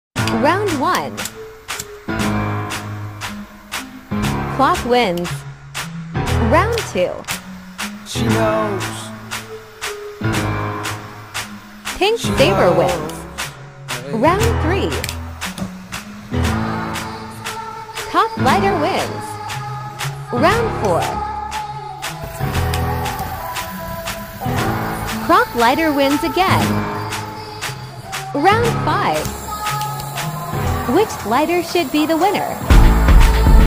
Battle of the Lighters 🔥
🔥 Lighters Based on Levels: Which One Blew Your Mind? 😳
Welcome to our YouTube Shorts video, where we unveil an extraordinary collection of lighters categorized into 6 mind-blowing levels! Which level left you in awe?

Get ready for an epic display of unique lighters, each representing a different level of awesomeness. From level 1 to level 6, the craziness keeps escalating!